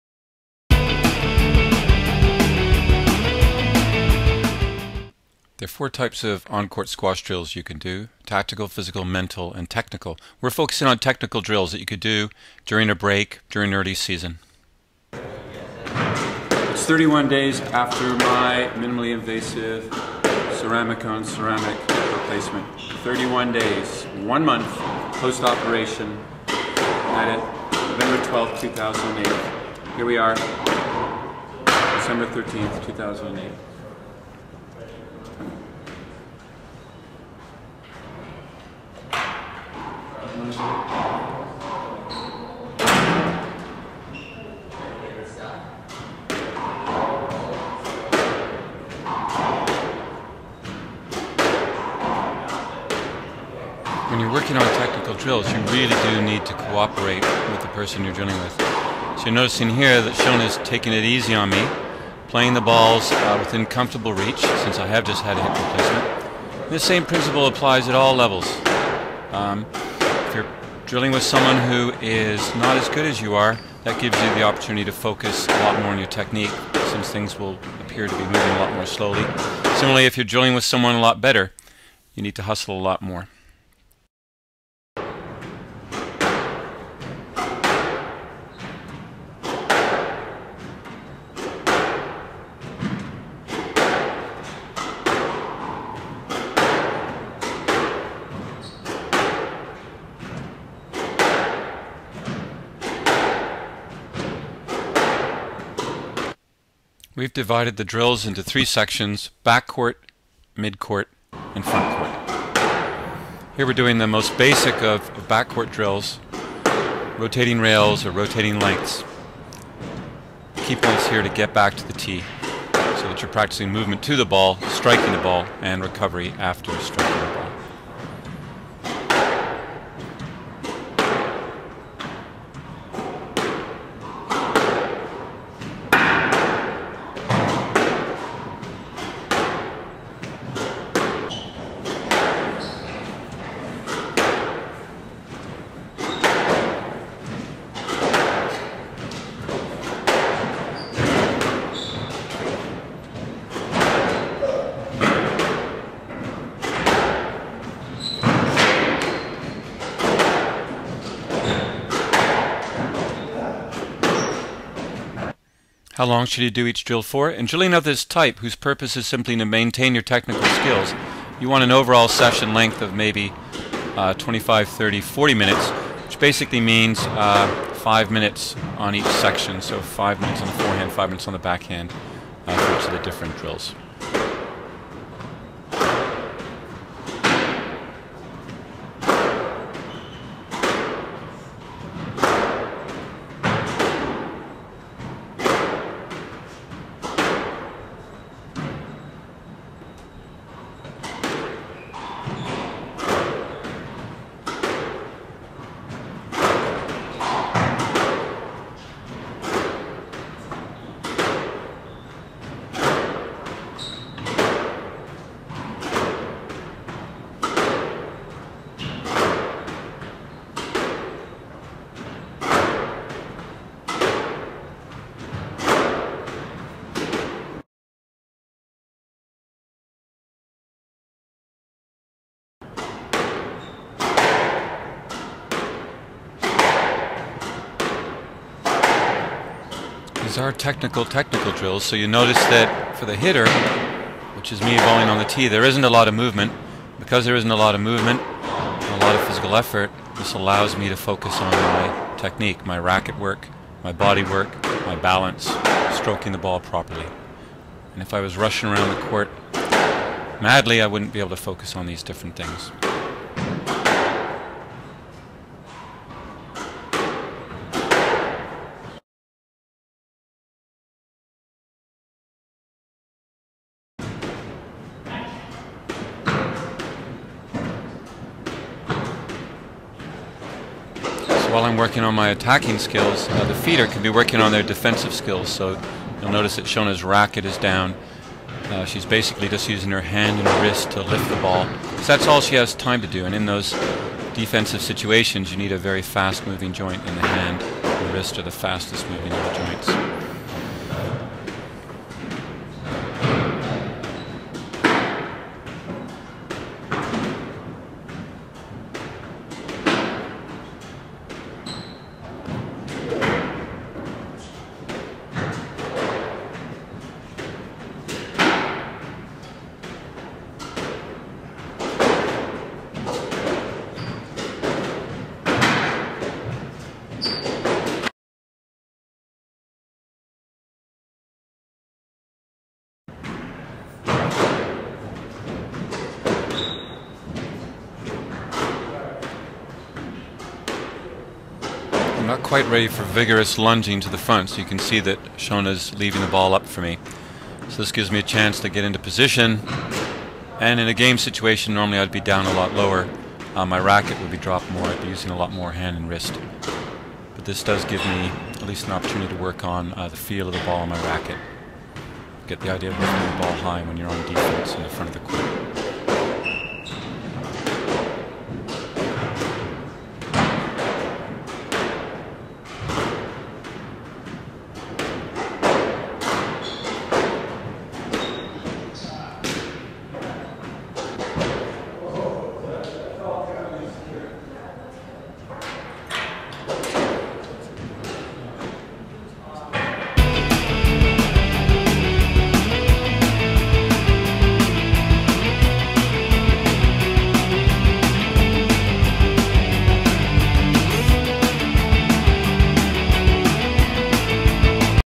Basic Squash Technical Drills
The most basic squash "technical" drills: back-court drives and boasts, mid-court volleys and drops, front-court lobs.  With Wesleyan Squash Coach (England Squash Level 3) Shona Kerr and Smith College Squash Coach Tim Bacon (Squash Canada Level 4) - 31 days after Tim's minimally invasive ceramic-on-ceramic total hip replacement (right) which he had on Nov. 12, 2008.